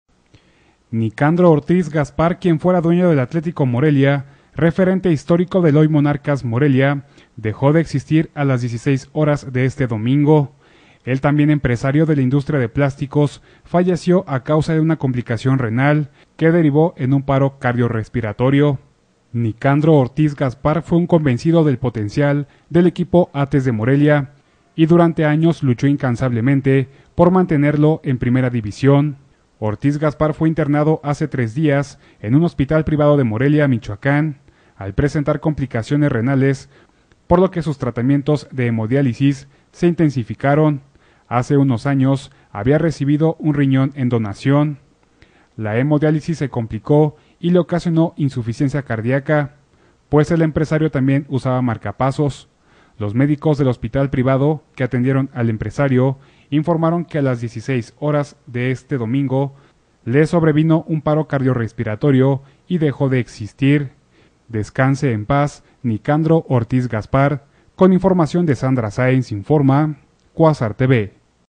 Muere Nicandro Ortíz Gaspar
Nicandro Ortíz Gaspar, quien fuera dueño del Atletico Morelia, referente histórico del hoy Monarcas Morelia, dejó de existir a las 16:00 horas de este domingo.